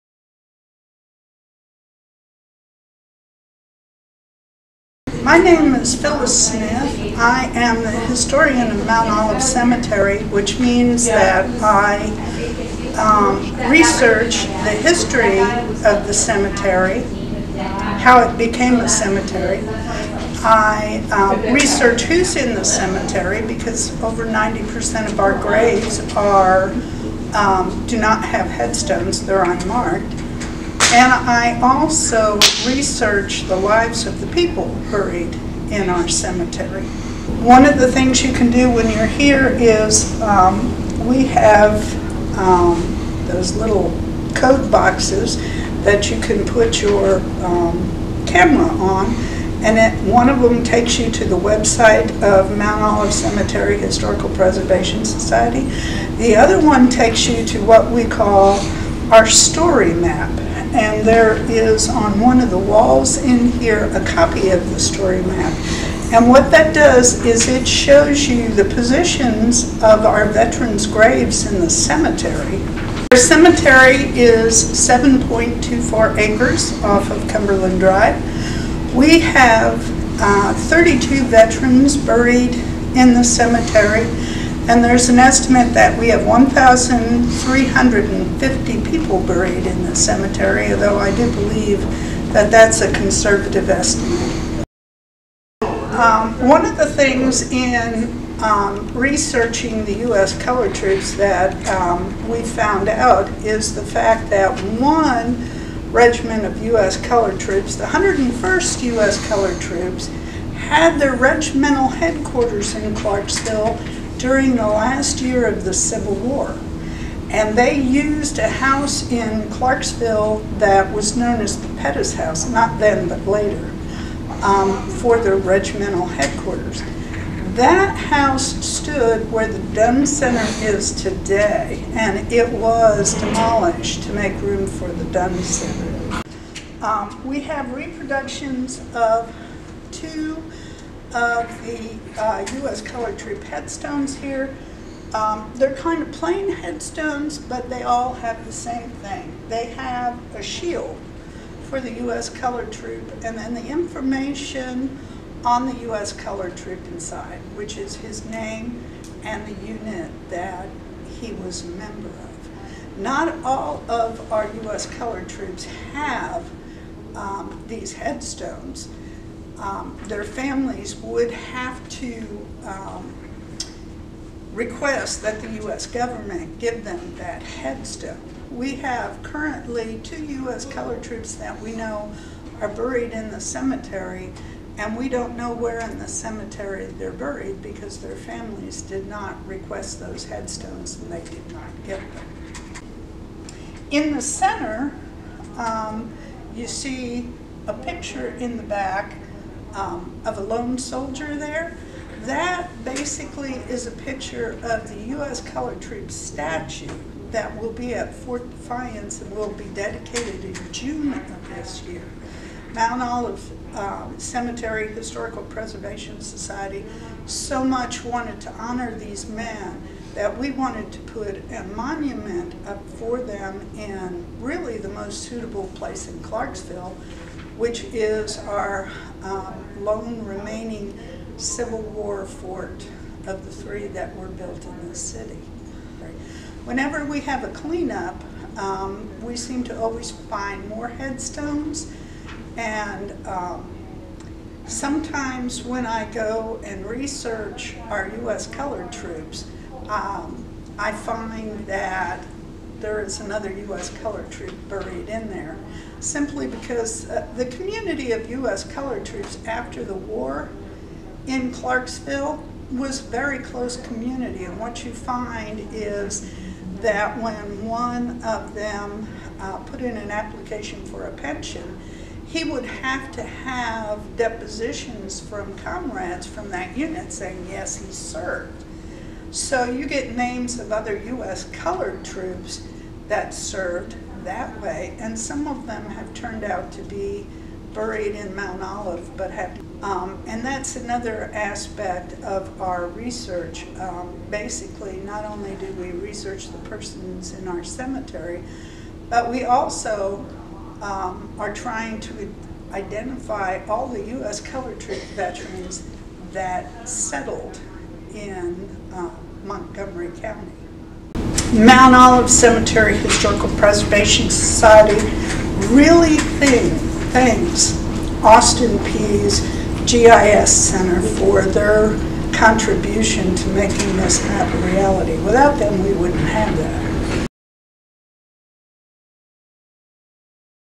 Mt. Olive Cemetery Historical Preservation Society
In recognition of Juneteenth, and to bring awareness to the United States Colored Troop Regiment during the Civil War, the Newton Military Family Resource Center partnered with Mt. Olive Cemetery. Mrs. Phillis Smith, historian at the Mt. Olive Cemetery Historical Preservation Society. In this short video, Mrs. Smith briefly touches on the history of the USCT as well as the actions the Historical Preservation Society has undergone to ensure these soldiers stories are told.